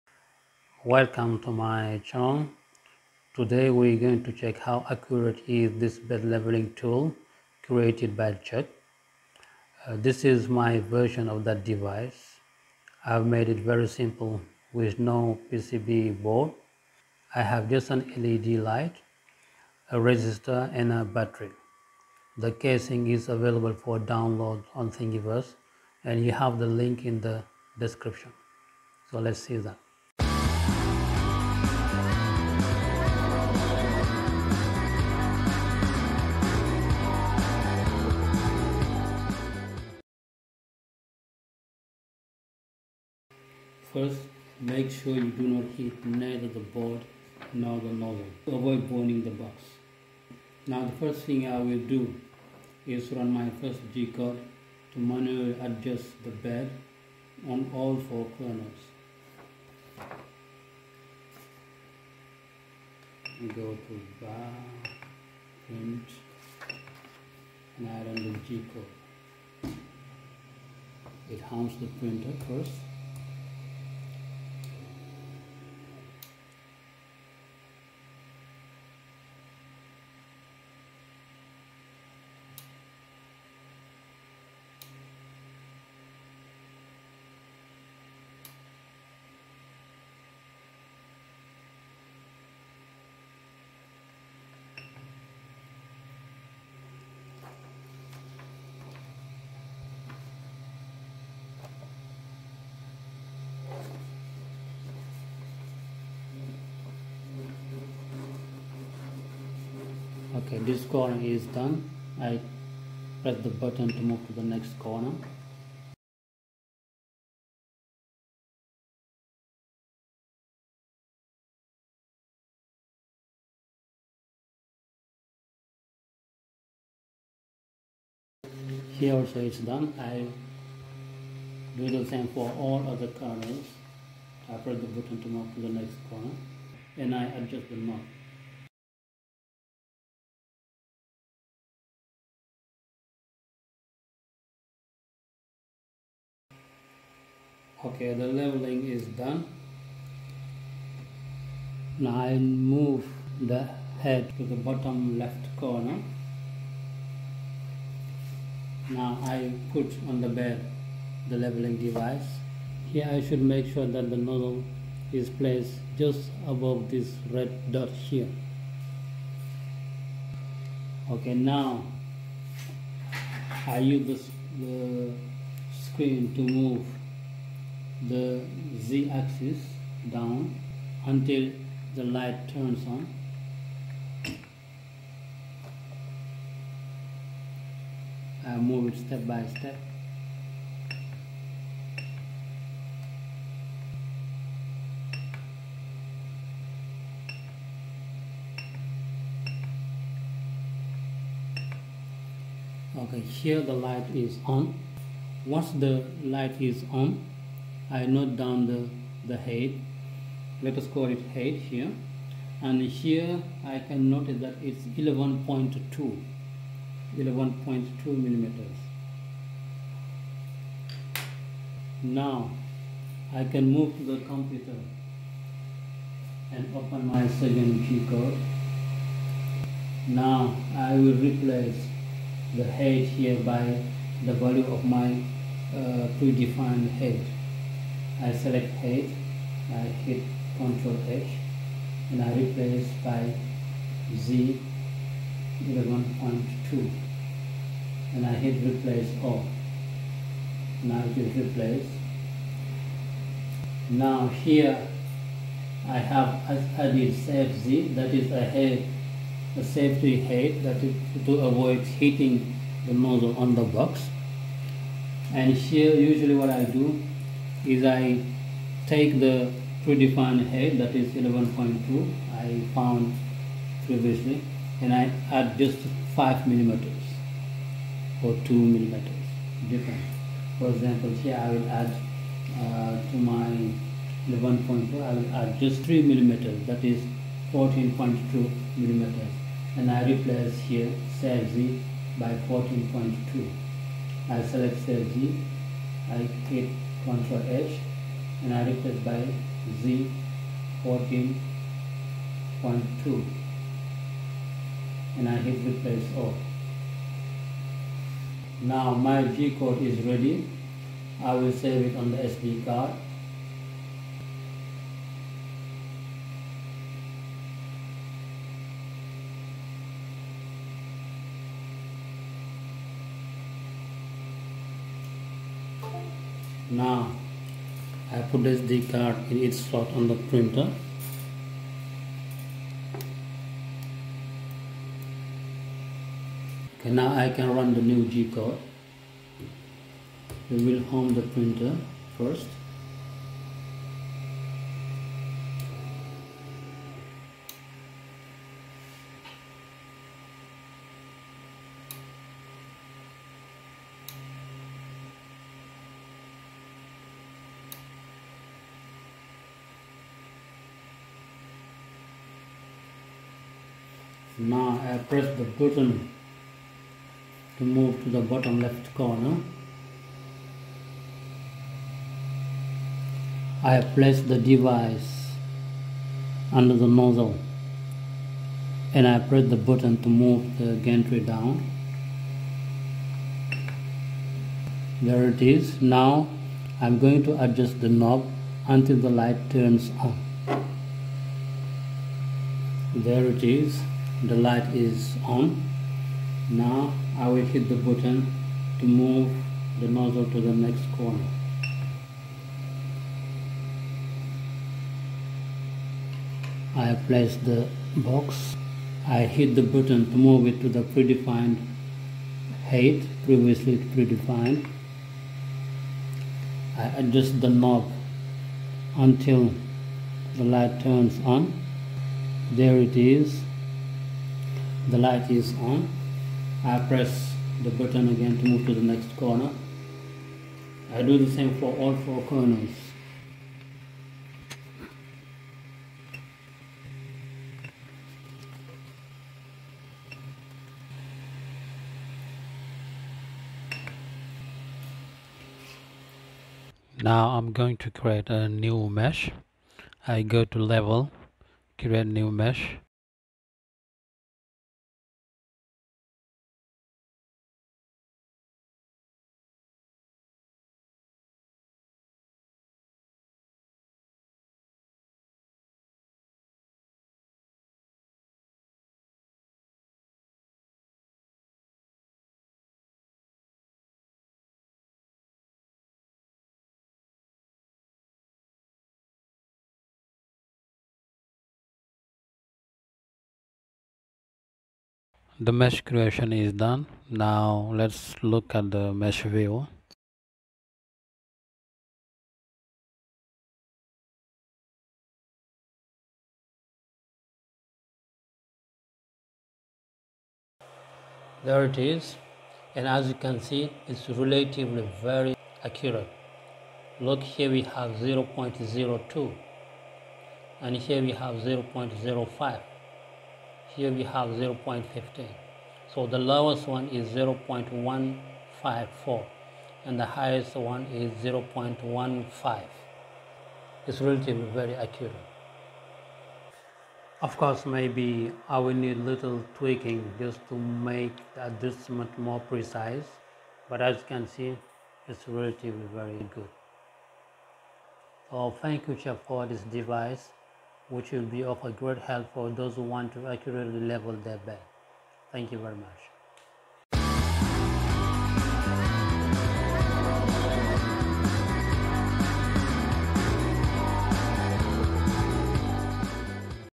73: How Accurate Is Chuck's Bed Leveling Tool (Subtitles: French & English)
In this video, I am testing a adapted version of Chuck's bed leveling tool. I will see how accurate it is.

===👇 To Get the Tool Kit 👇===
CHEP Electronic Bed Leveling Tool Kit

===👇 To Watch the Video by CHEP 👇===

- From AliExpress
- From Amazon

- From AliExpress
- From Amazon

- ✍Send me your suggestions.